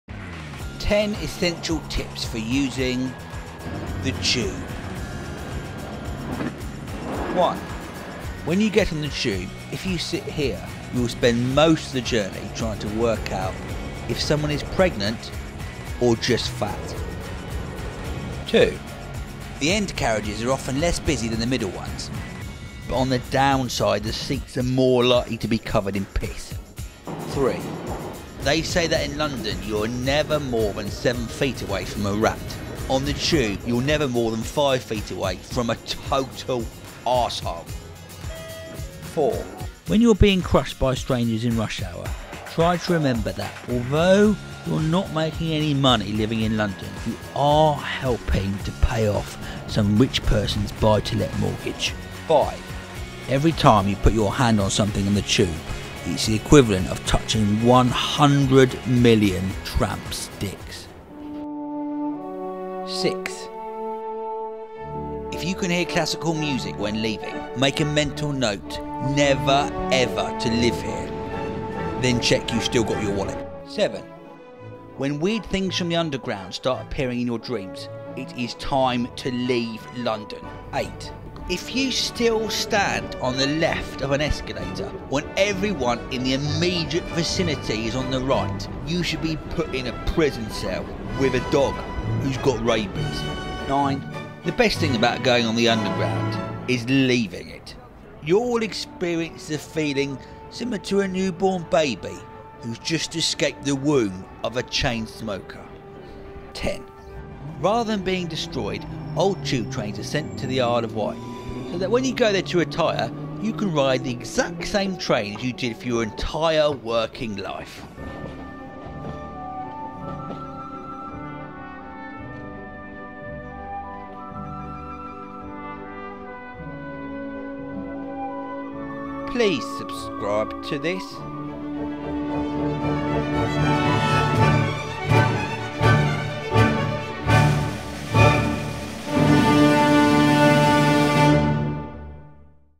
10 Essential Tips for Using the Tube : London Underground
Marek Larwood gives ten top tips he has learnt from using the London Underground.